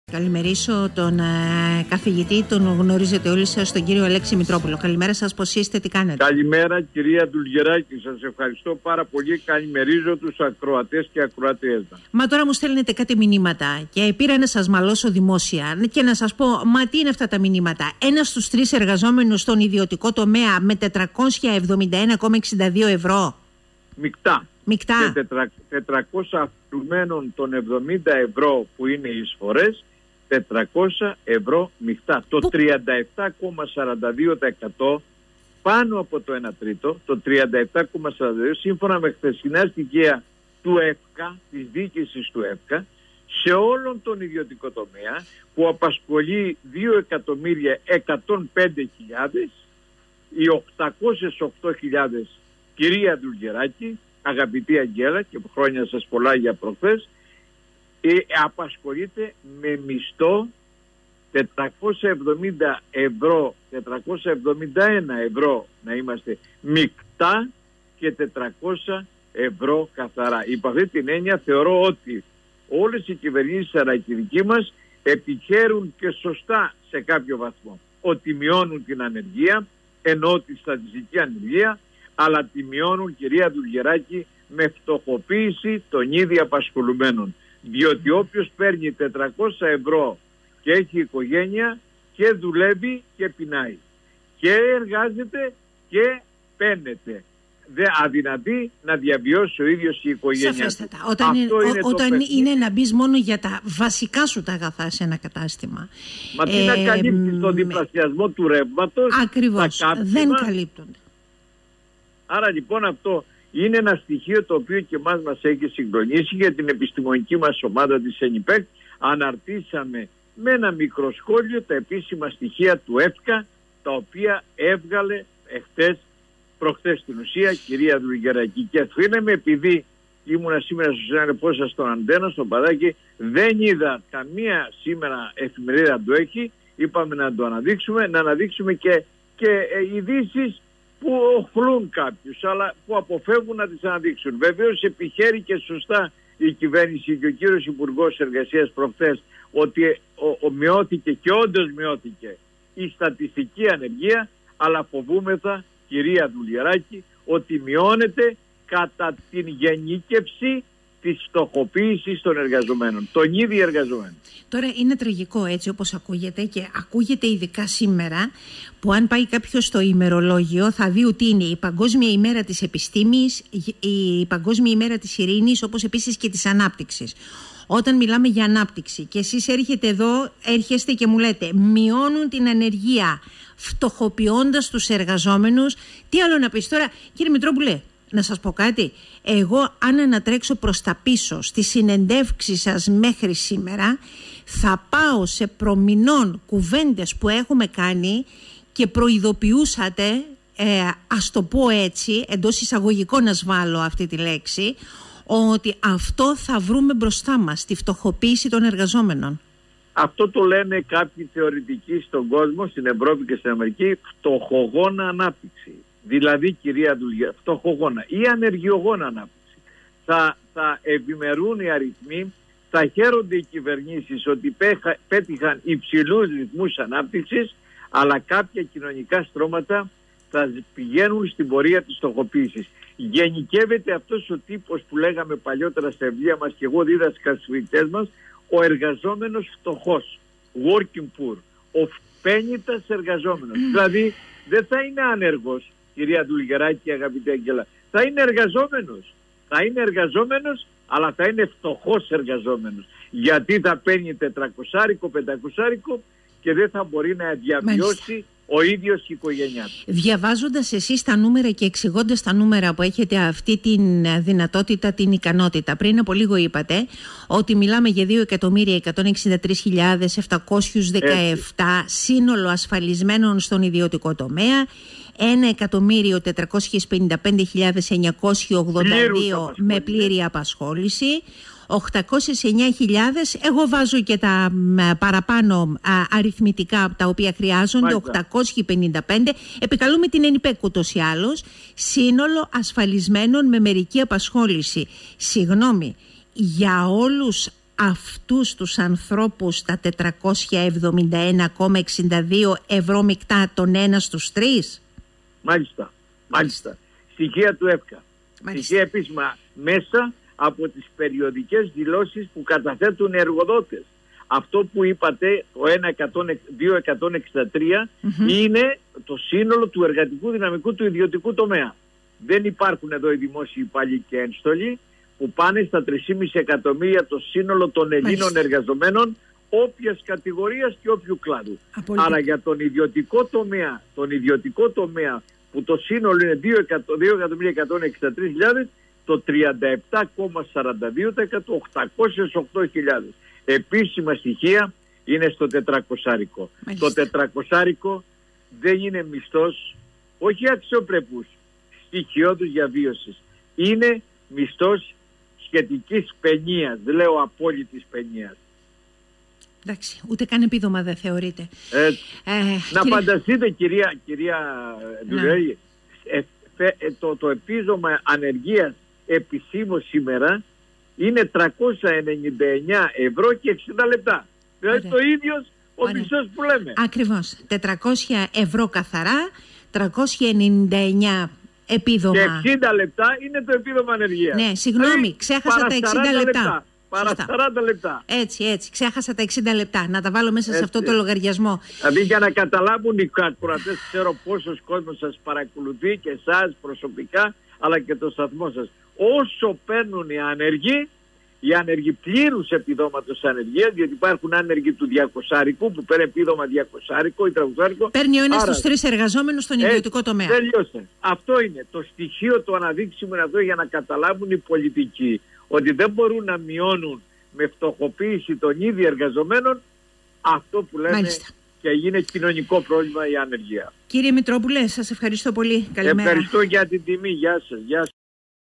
Α. Μητρόπουλος: Στα 400 ευρώ ο μισθός για το 37,42% των εργαζομένων στον Ιδιωτικό Τομέα
«Μειώνουν την ανεργία δημιουργώντας φτωχούς εργαζόμενους»

1 στους 3 εργαζόμενους στον ιδιωτικό Τομέα αμείβεται με 471,62 ευρώ μικτά – 400 καθαρά, με αποτέλεσμα την έκρηξη του αριθμού των φτωχών εργαζόμενων , όπως λέει στον 98.4 ο καθηγητής Αλέξης Μητρόπουλος  επικαλούμενος τα στοιχεία του ΕΦΚΑ.
Μιλά για έκρηξη των φτωχών εργαζόμενων με μισθό ίσο του επιδόματος ανεργίας που ανέρχεται στα 399,60 ευρώ και προσθέτει ότι μιλάμε για σχέδιο βίαιης αναδιανομής του πλούτου σε βάρος της εργατικής τάξης.
Τα στοιχεία του ΕΦΚΑ αφορούν στο Μάϊο του 2020 και προέρχονται από την επεξεργασία των περιοδικών δηλώσεων για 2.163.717 εργαζόμενους από τους οποίους οι :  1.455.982 με πλήρη απασχόληση και οι 809.855με μερική.
Ο μέσος μισθός μεικτά για τους πλήρους απασχόλησης ανέρχεται στα 1.203,42 ευρώ και για τους εργαζόμενους μερικής απασχόλησης στα 471.62 ευρώ μικτά.